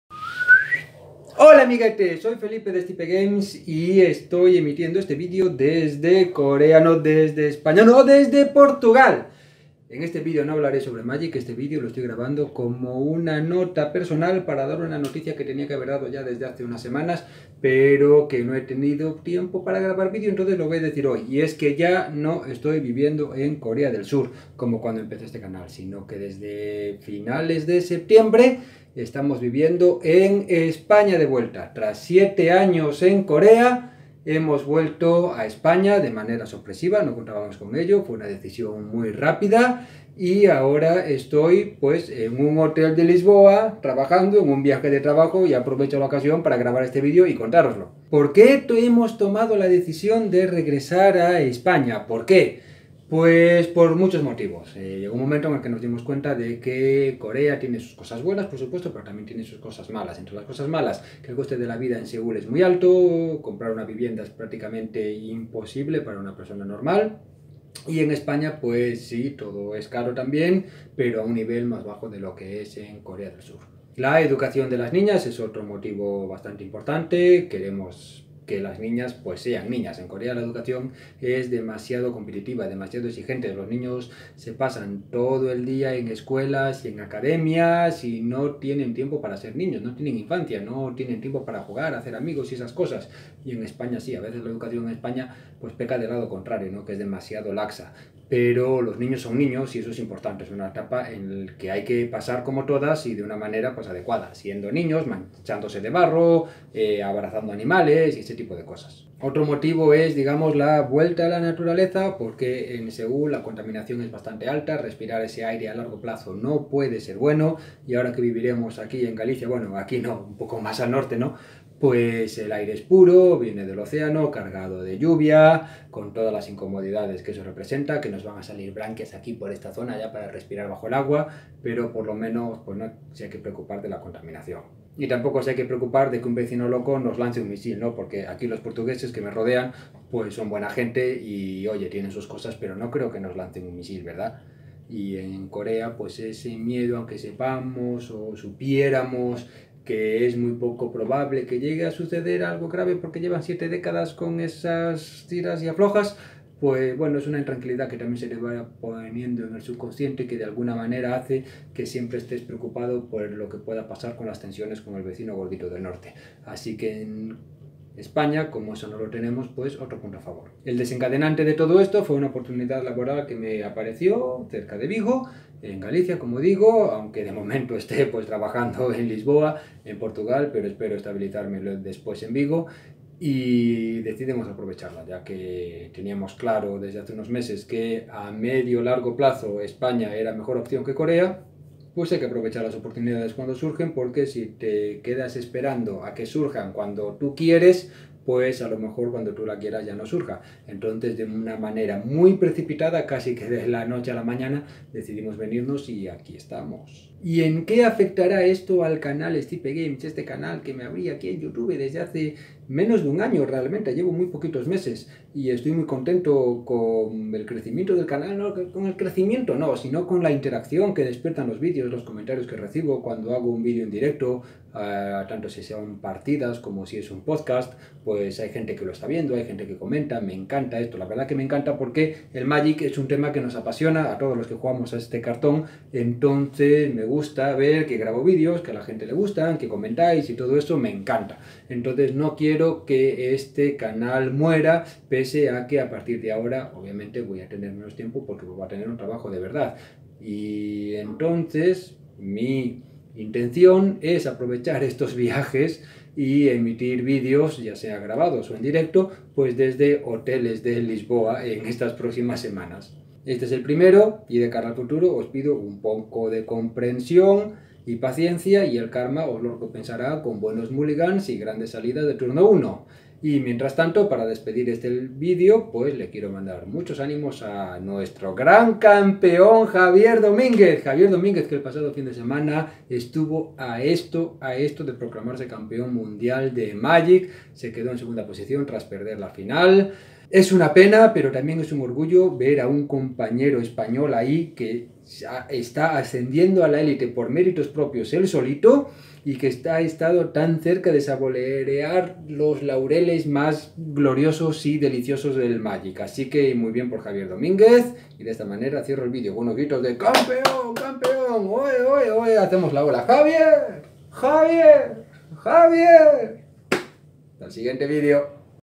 Cambiamos las Coreas por las Españas
Después de siete años viviendo en Corea, me he mudado de vuelta a España. En el vídeo explico los motivos y lo que haré en el futuro inmediato en España. De momento por motivos laborales estaré pasando mucho tiempo en Portugal, casi todas las semanas de lunes a viernes, pero espero estabilizarme en Galicia pronto.

Mira mi lupa para detectar cartas falsas
Y mi micro
Y mi webcam
Y mi pantalla
Y mi ratón
Y mis auriculares cutres